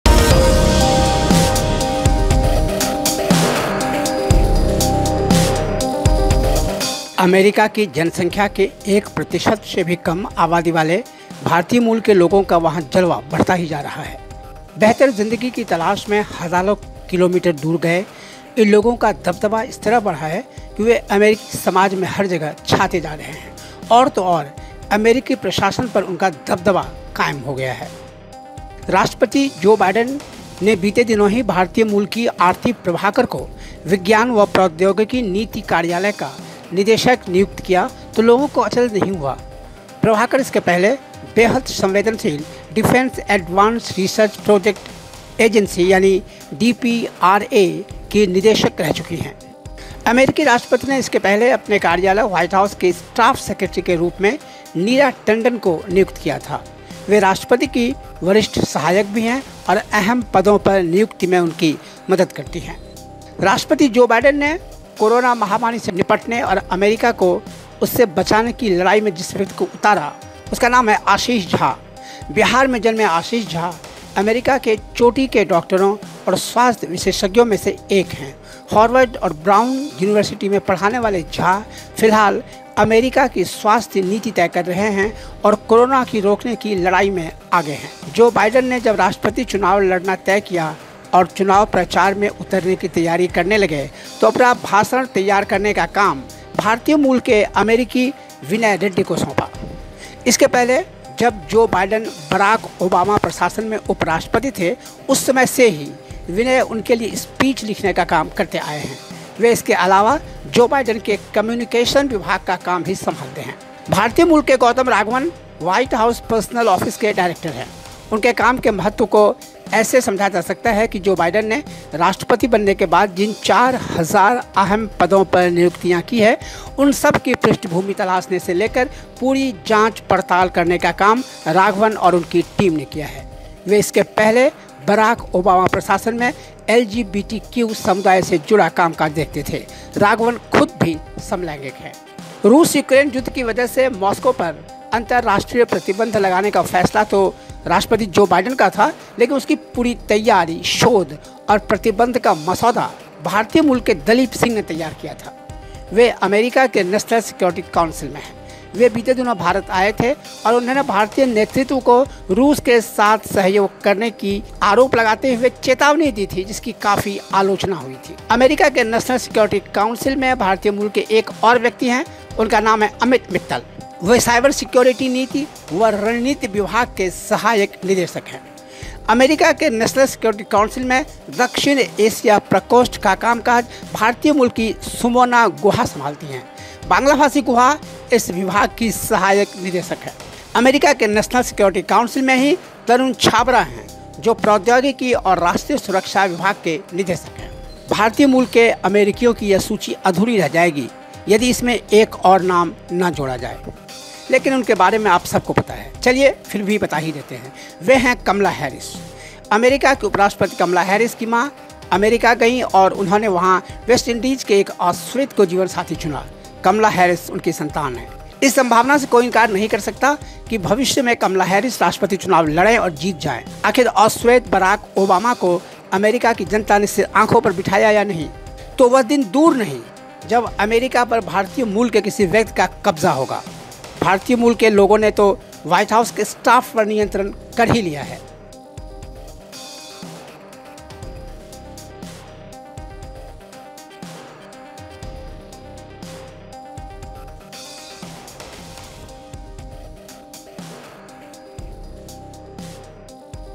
अमेरिकी प्रशासन में अहम पदों पर कैसे पहुंचे भारतीय मूल के लोग?
अमेरिका की जनसंख्या के एक प्रतिशत से भी कम आबादी वाले भारतीय मूल के लोगों का वहां जलवा बढ़ता जा रहा है। बेहतर जिंदगी की तलाश में हजारों किलोमीटर दूर गए इन लोगों का दबदबा इस तरह बढ़ा है कि वे अमेरिकी समाज में हर जगह छाते जा रहे हैं। और तो और, अमेरिकी प्रशासन में उनका दबदबा कायम हो गया है।